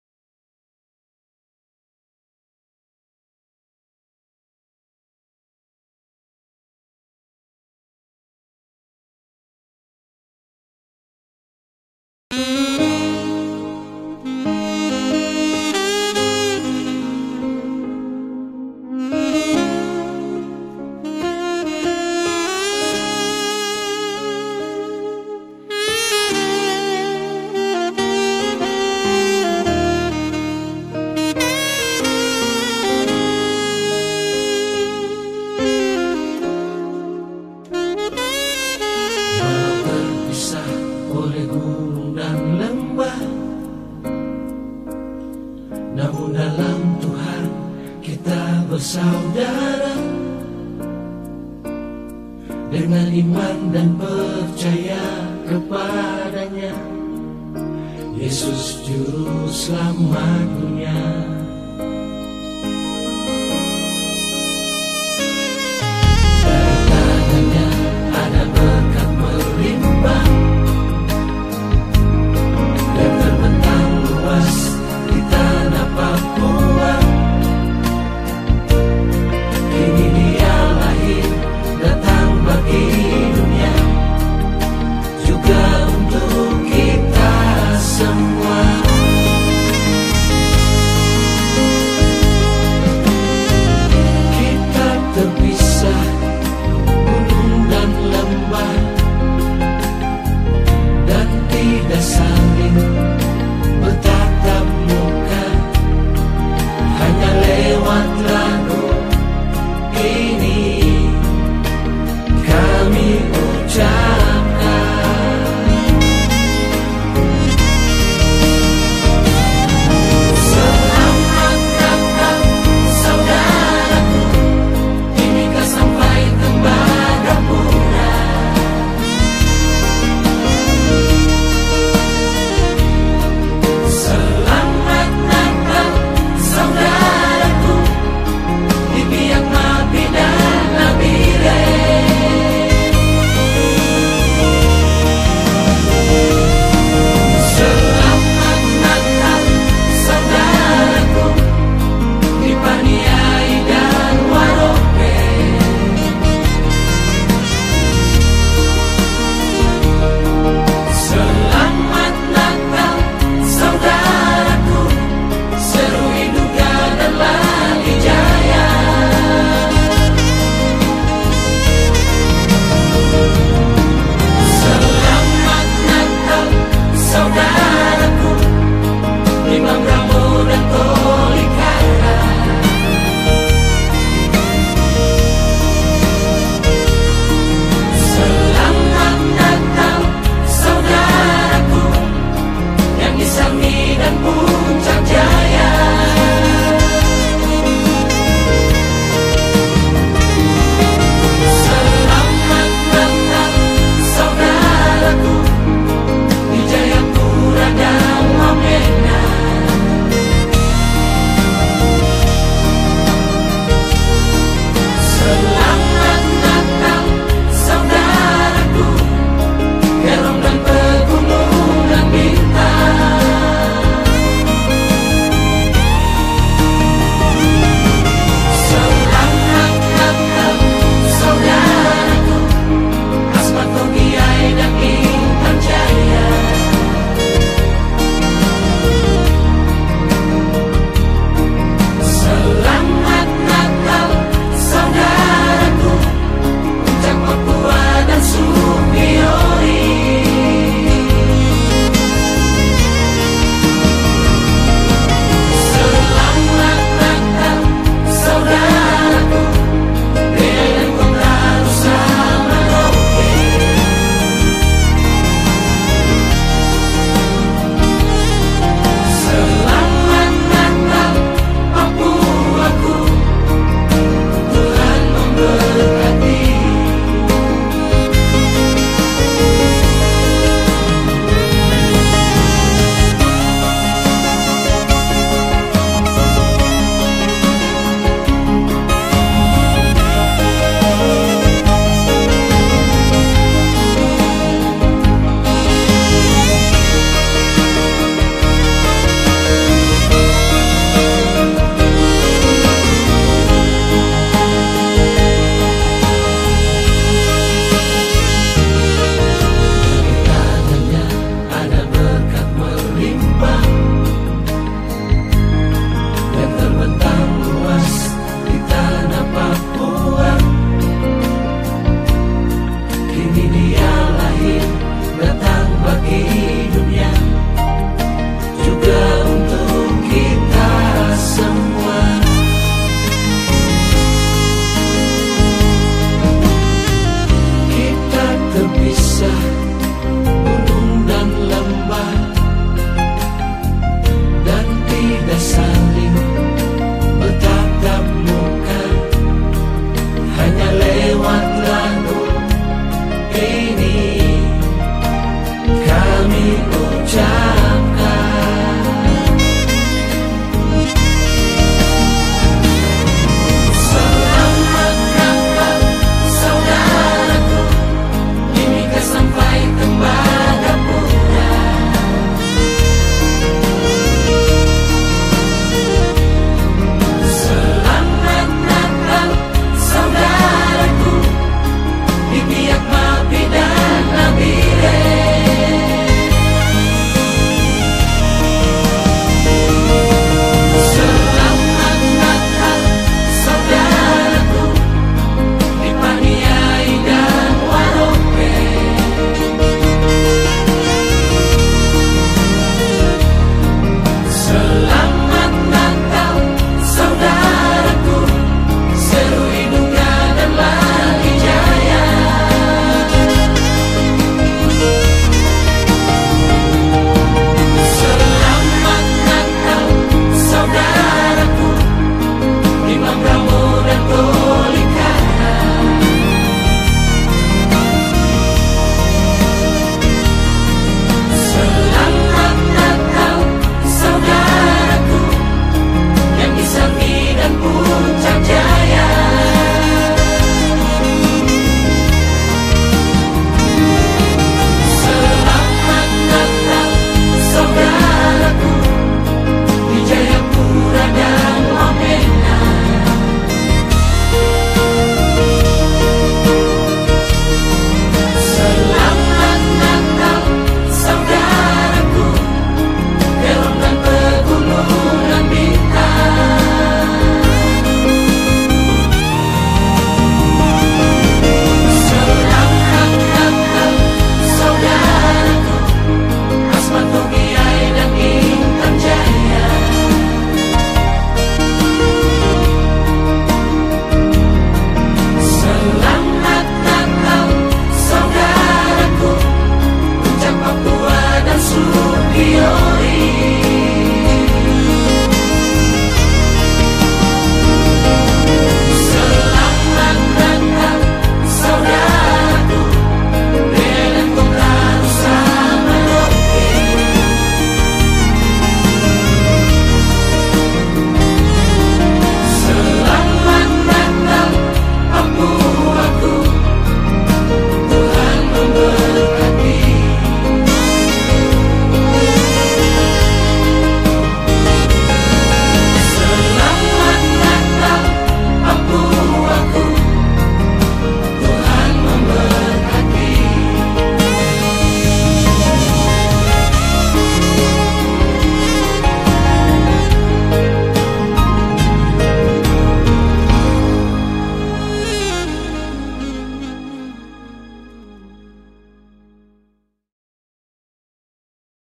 LAGU NATAL PALING SEDIH 2021 BPK.KLEMEN TINAL.SE.MM
PERJALAN PANJANG SEORANG MANTAN KARYAWAN PT.FI,BUPATI MIMIKA & WAKIL GUBERNUR PAPUA...